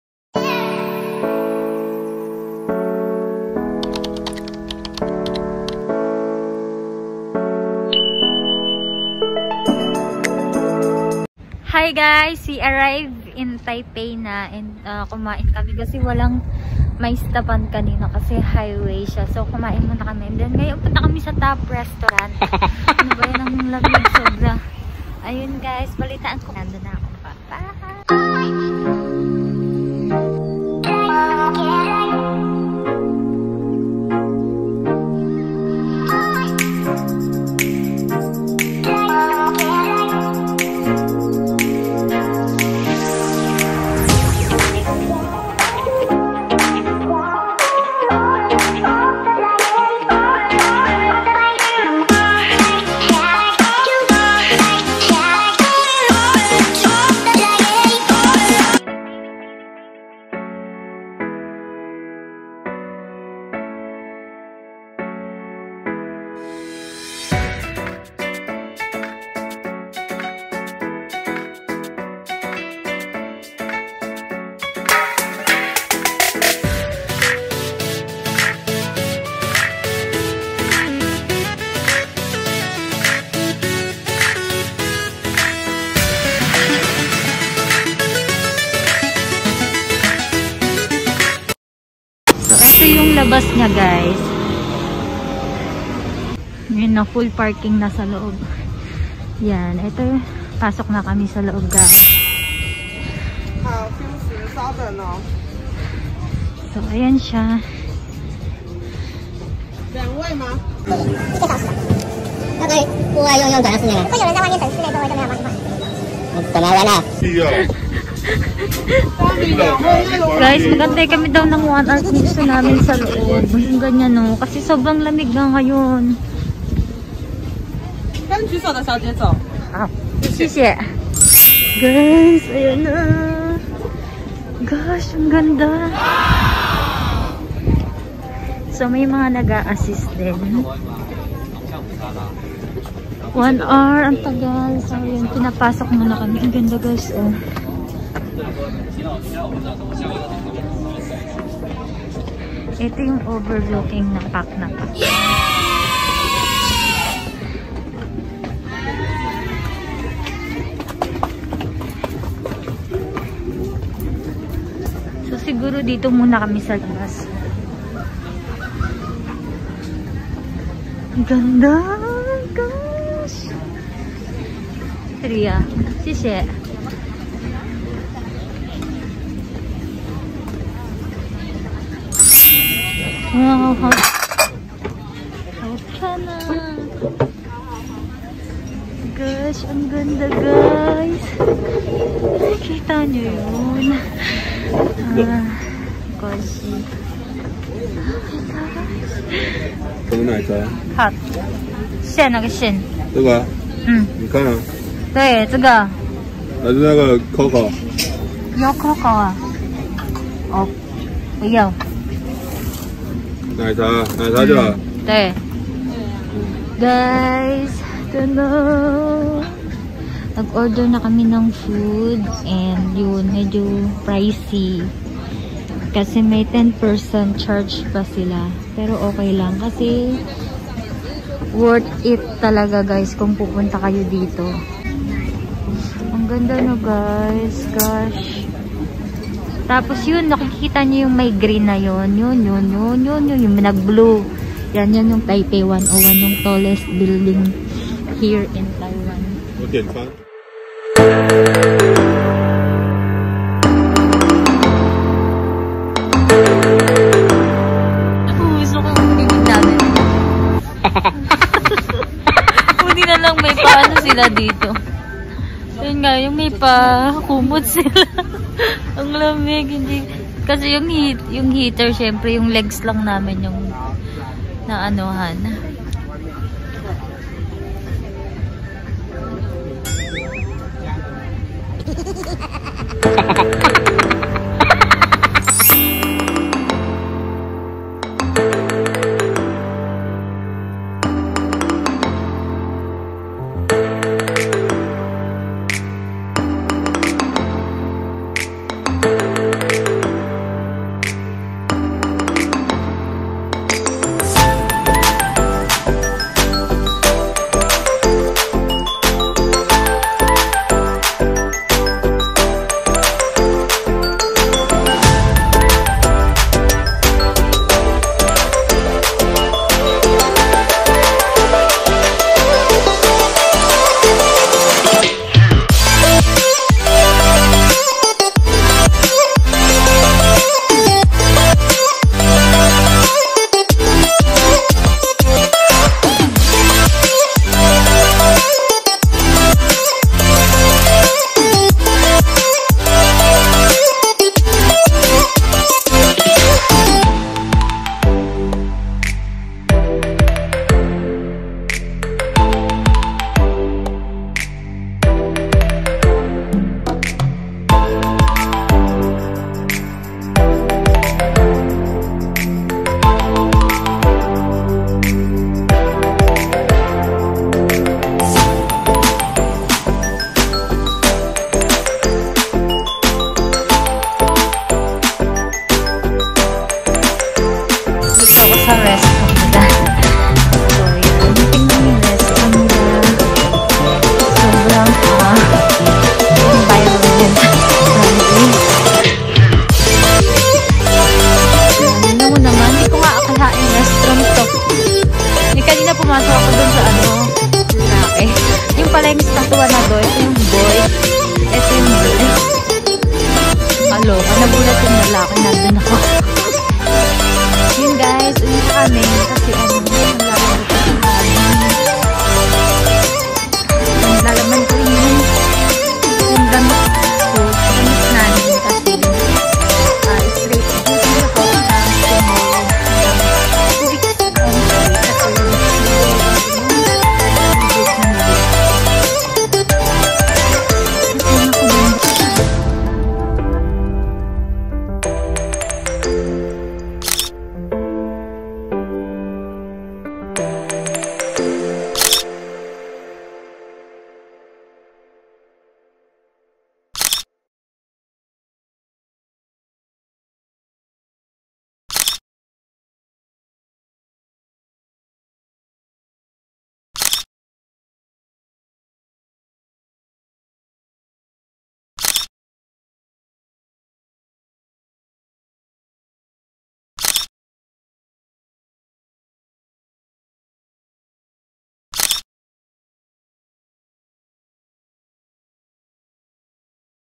THE TOP RESTAURANT | BEST OVERLOOKING RESTAURANT IN TAIPEI, TAIWAN
Planning to have a trip in Taiwan (Taipei)? You might include this in your bucket list. Perfect for any occasions. Most especially this coming Valentines day. So here's your guide.

📍HOW TO GET THERE:
🚇 From TAIPE MAIN STATION RIDE MRT GOING TO MINGDE (REDLINE)
🚖 from MINGDE RIDE A TAXI GOING TO THE RESTAURANT (taxi fare NT$240) show the chinese address below👇🏻

ALTERNATIVE WAY VIA:
🚌 BUS#260 GOING TO SHANZAIHOU POLICE STATION stop there and ride a taxi
📍THE TOP ADDRESS:
Chinese Address: The Top 屋頂上
台北市士林區凱旋路61巷4弄33號(文化大學體育館下方50公尺處)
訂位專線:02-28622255

📍English:  The Top 屋頂上
Kaixuan Shihlin District No. 33, Lane 61, Alley 4 (Culture University Gymnasium below 50 meters)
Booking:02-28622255

⏰WORKING HOURS:
Mon-Fri  17:00-3:00am
Saturday  12:00-3:00am
Sunday 12:00-3:00am
Public holiday 12:00-3:00am

💸Minimum charges:
🧷Outdoor is NT$350 per person
🧷Villas Minimum Charges:
🧷BOX VILLA C&D = NT$3000
🧷BOX VILLA E = NT$6000
🧷VIP VILLA = NT$8000

NO ENTRANCE FEE BUT NT$350 MIN. OF ORDER + 10% SERVICE FEE.

IMPORTANT NOTES/ADVICE:
🗓 PAYMENT POLICY: CASH ONLY (NO CREDIT/DEBIT CARDS)
🗓 GO THERE EARLY AS POSSIBLE, THE RESTAURANT IS TOO FAMOUS, SO EXPECT LONG LINES.
🗓ABOUT FOOD: THEY HAVE BBQ, HOTPOT, DRIED FOOD, LIGHT AND HEAVY MEALS THEY ALSO HAVE ALCOHOL DRINK and LAST ONE:

📸 BRING YOUR CAMERA AND ENJOY!